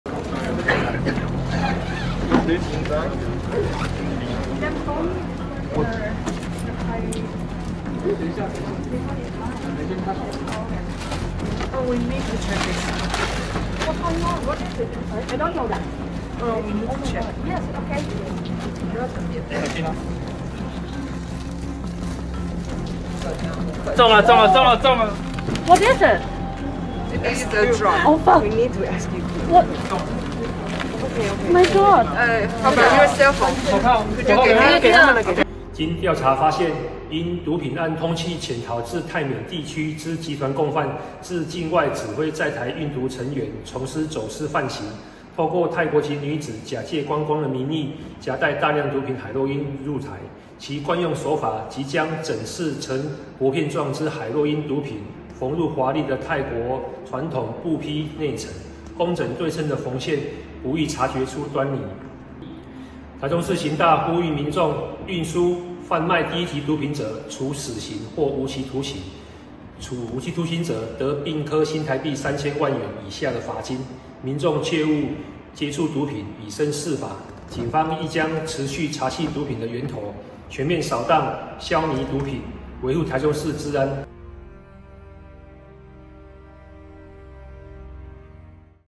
走私新招！上億海洛因縫入泰國桌旗 泰女夾帶闖關被活逮（警方提供）
走私新招！上億海洛因縫入泰國桌旗 泰女夾帶闖關被活逮

泰國籍萬姓女子接受境外毒梟指揮，將海洛因毒品打碎壓縮成薄片狀，縫在泰國傳統桌旗布飾內企圖夾帶走私來台，檢警去年7月中旬在機場守株待兔，趁萬女入境通關時逮人，起出黑市價格高達上億元的13公斤海洛因，檢方聲押萬女獲准，全案近日偵結起訴，法院裁定接押。